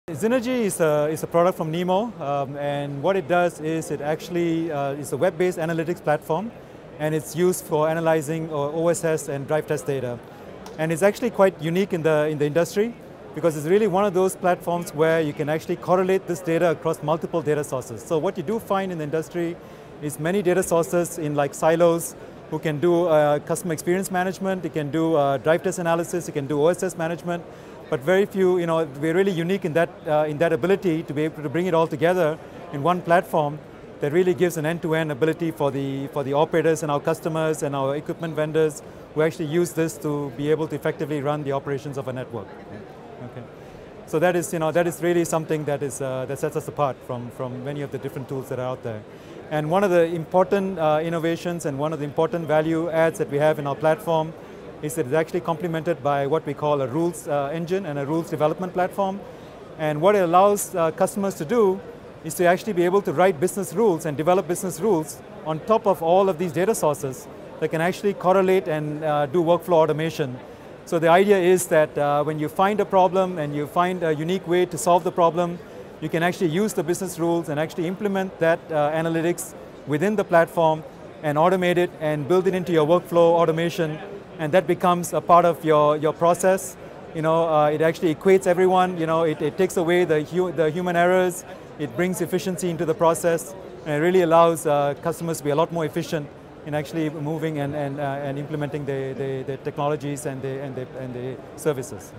MWC15: Anite Network Testing explains Xynergy System Modules
Ashok Sethu, Vice President of analytics at Anite Network Testing, explains the company's new web-based analytics platform, Xynergy System Modules at Mobile World Congress 2015.